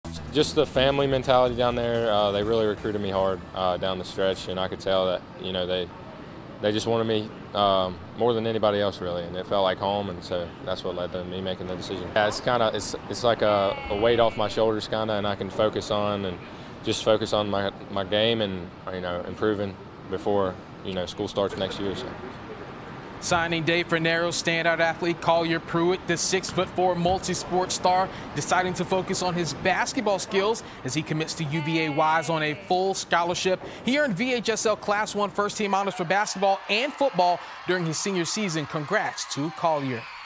Narrows' Pruett commits to UVA-Wise
Kolier Pruett signing ceremony at Narrows High School